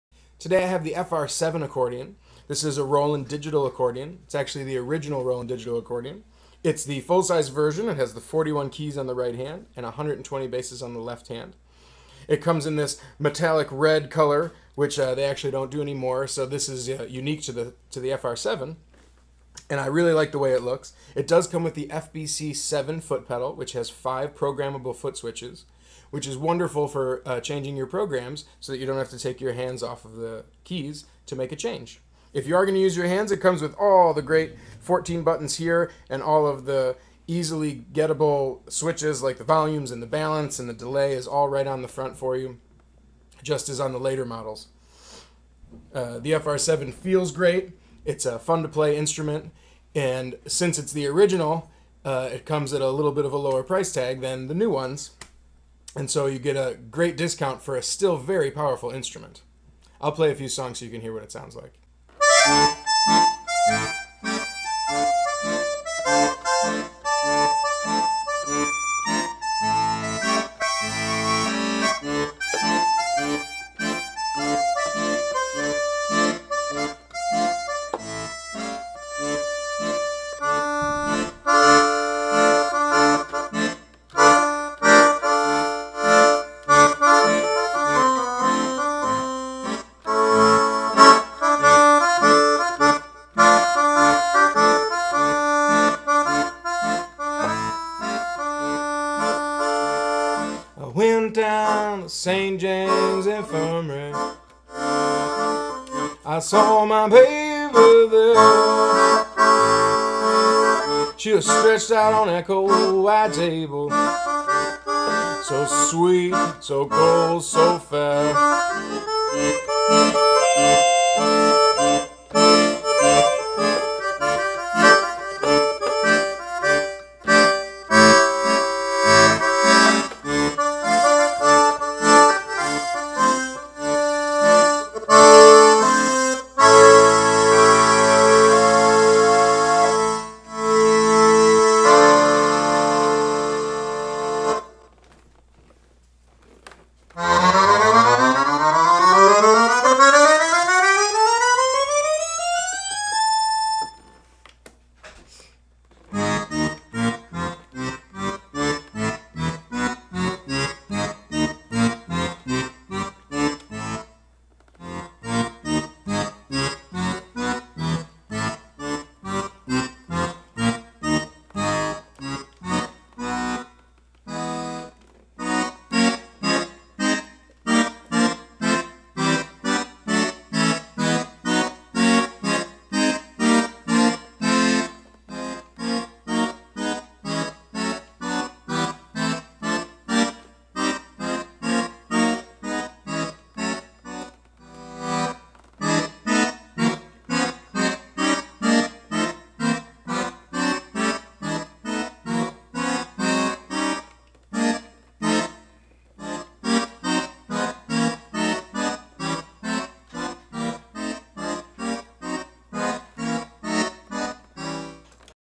2315 - Red Roland FR-7 Digital Piano Accordion 41 120 $1995
Red Roland FR-7 Digital V-Accordion
Piano Accordion 19.25" 41/120 25lbs
Reedless full size digital accordion
Top condition with foot pedal, cord, box, manual
Includes Straps and Roland Gig Bag